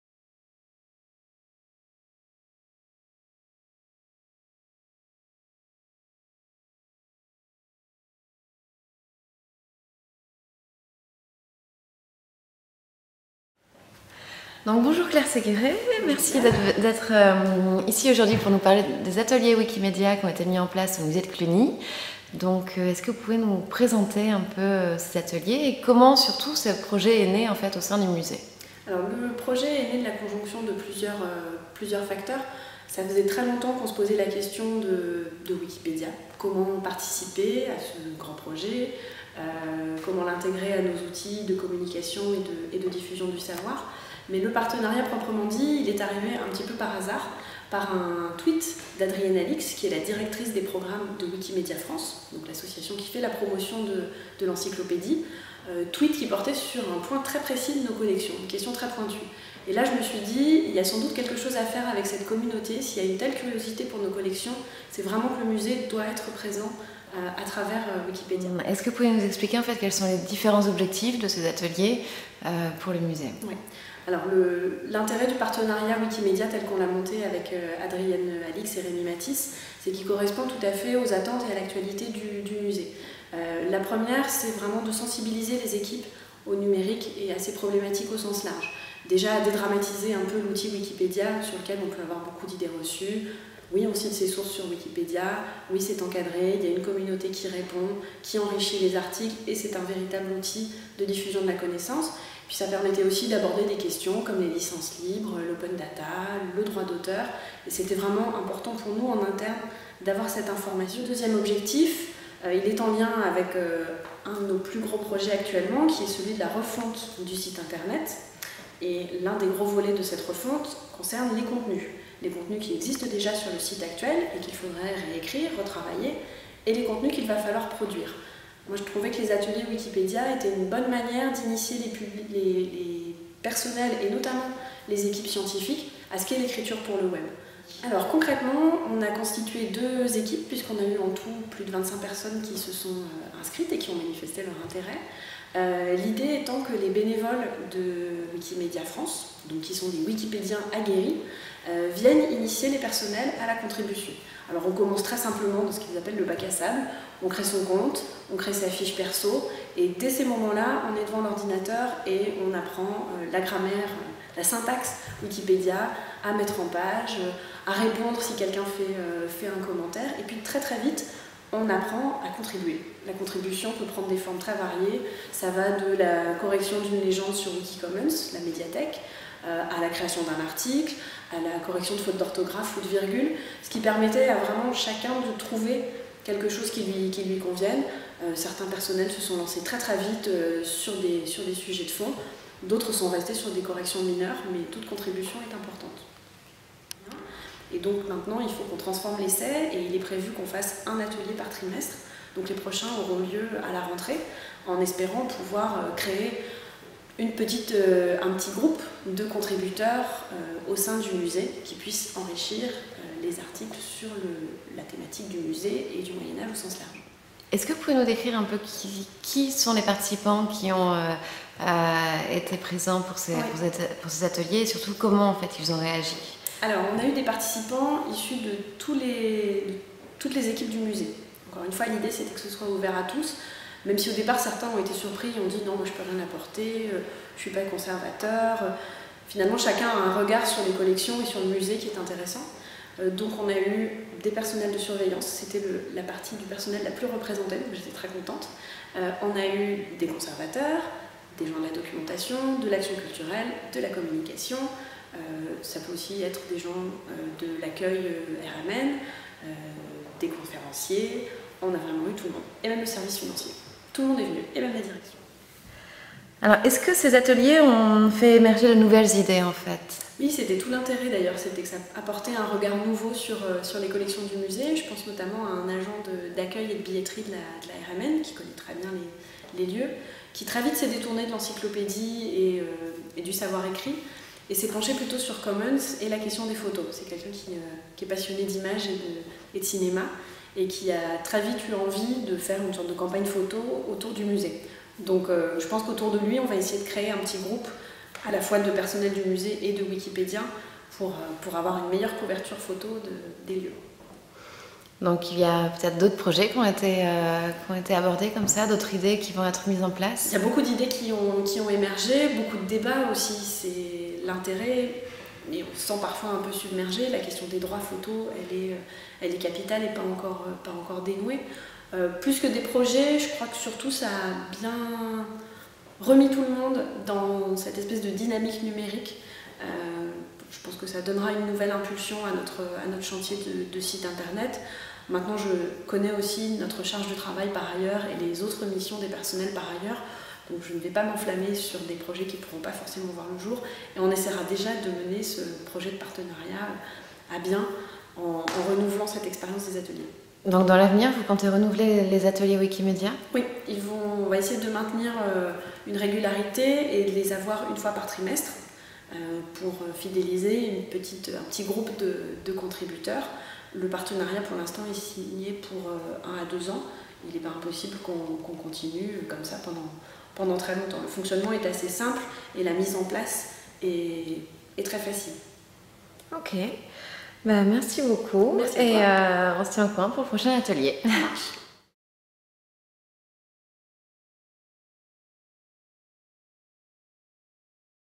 Ateliers Wikipédia au musée de Cluny (2012)
Interview de Claire Séguret, responsable adjointe de la communication et du mécénat, à propos des ateliers Wikipédia organisés au musée de Cluny. Interview menée par Diane Drubay pour Buzzeum.